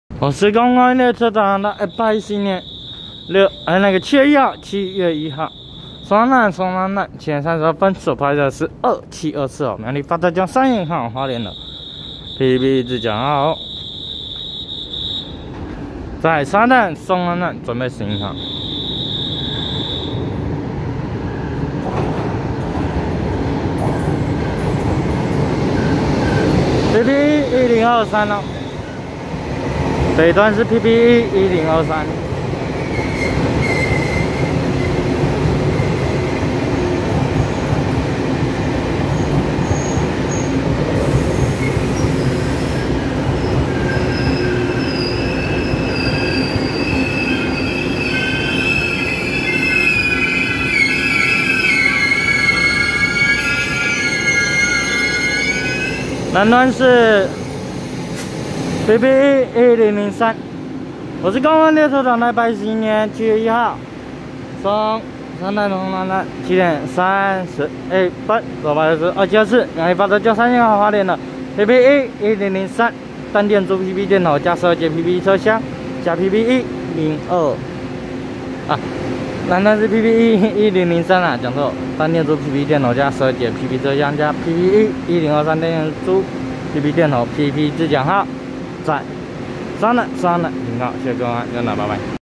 272次苗栗發車經由山線開往花蓮的PPE1003單電豬PP電頭+十二節PP車廂+PPE1023單電豬PP電頭PP自強號
111/7/1松山站7:32分拍攝272次苗栗發車經由山線開往花蓮的PPE1003單電豬PP電頭+十二節PP車廂+PPE1023單電豬PP電頭PP自強號